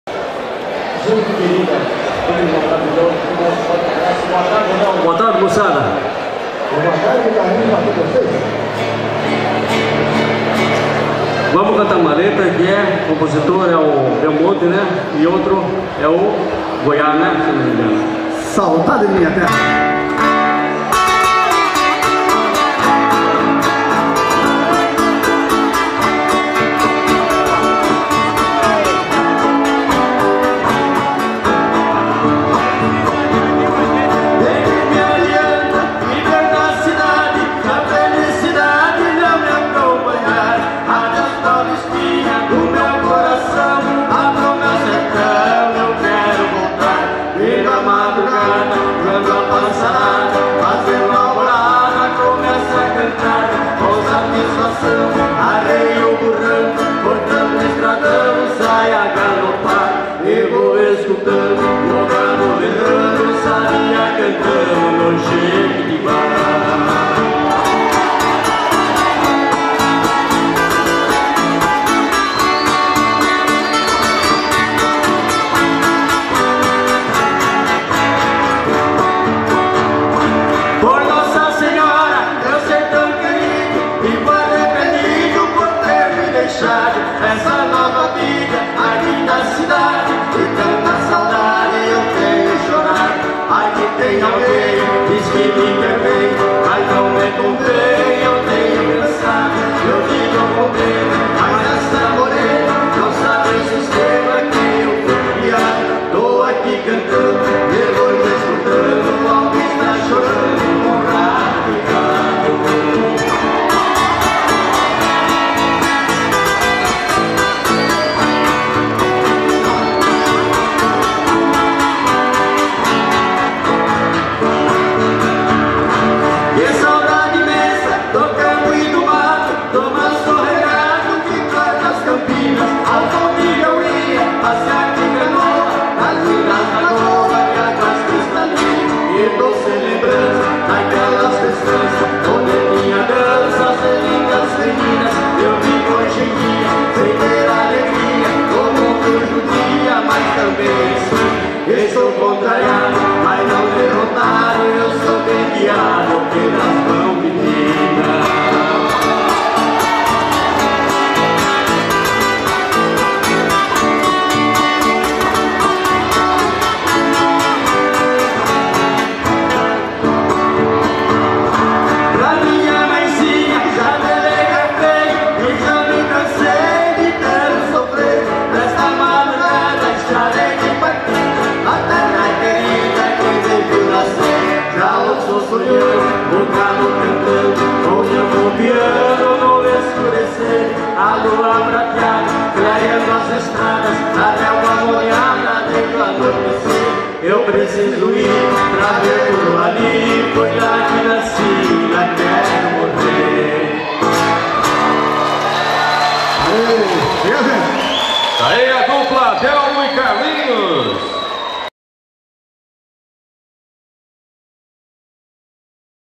Delmo e Carlinhos.f4v
Delmo e Carlinhos na 40º Roda de Violeiro Ferminio Gonçalves de Freitas dia 1 de maio de 2012 em Registro, realização Prefeitura Municipal de Registro.